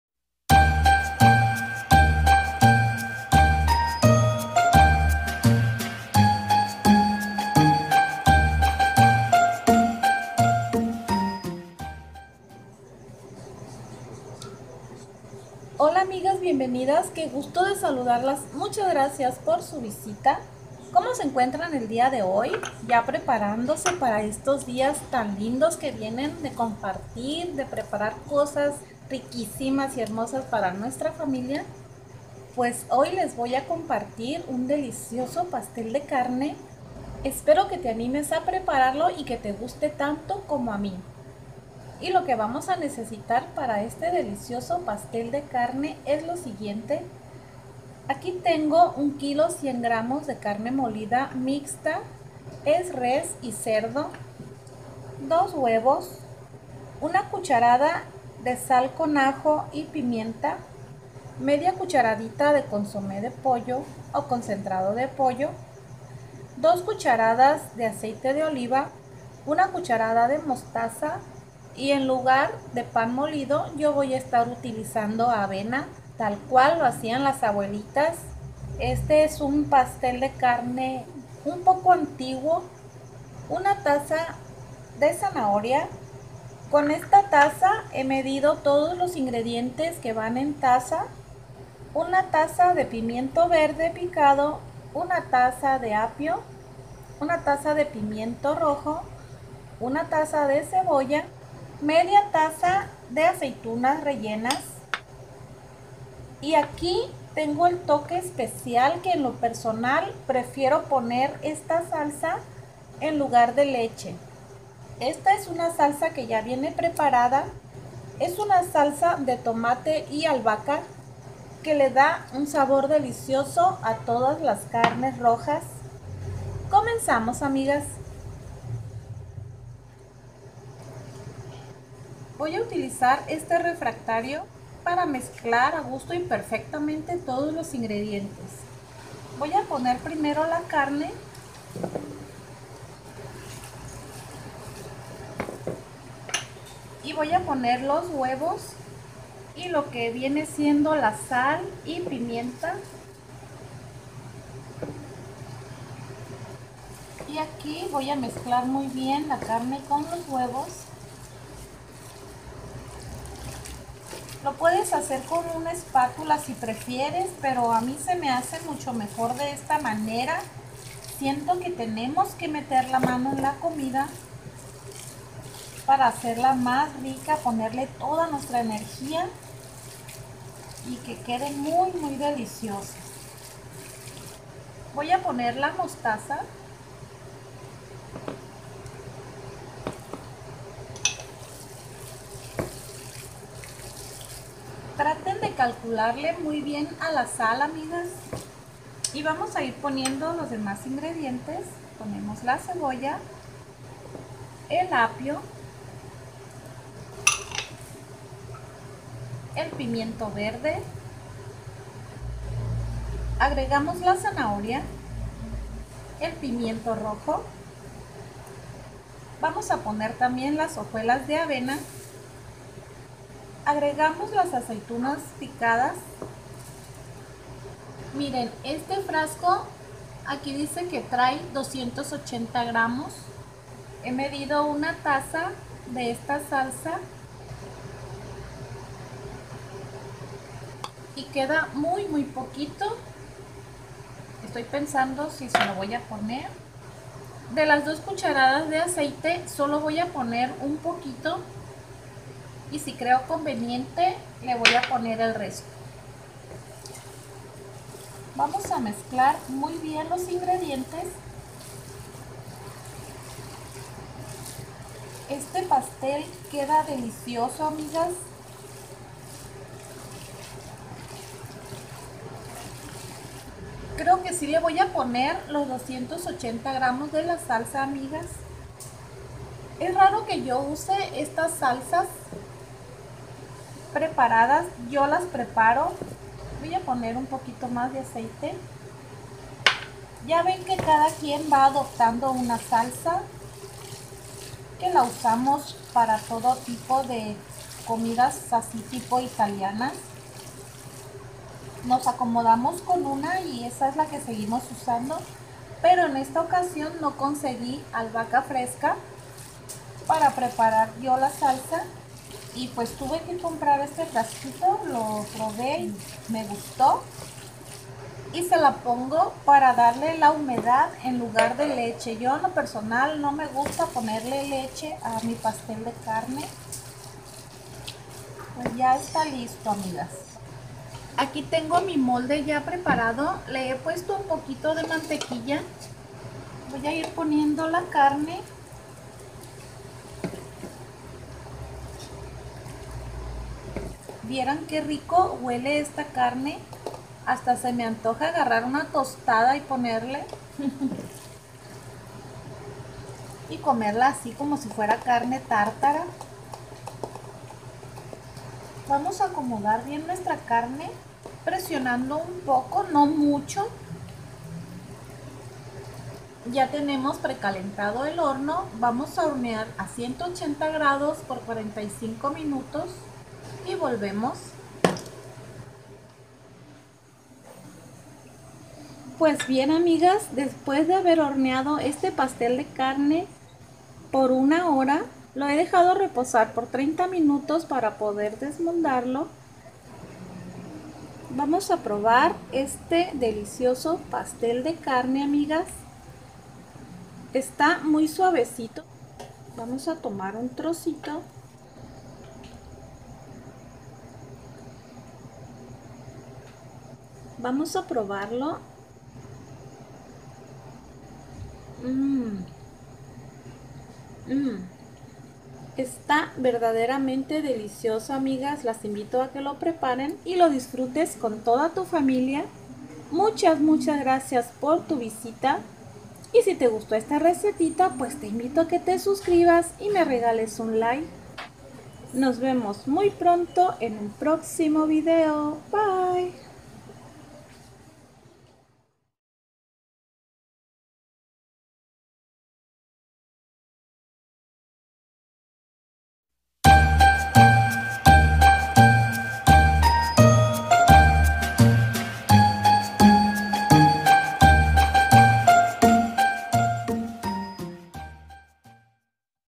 Pastel de carne!!
1,100 gr.de carne mixta(res y cerdo)
1 taza de pimiento verde
1 taza de pimiento rojo
1 taza de zanahoria
1 taza de apio
1 taza de cebolla blanca
1 taza de avena
1/2 taza de aceitunas
2 cdas.de aceite de oliva
1 cda.de mostaza
1/2 cda.de concentrado de pollo
Una cda.de sal de ajo con pimienta molida
Salsa de tomate con albahaca de 280 gr.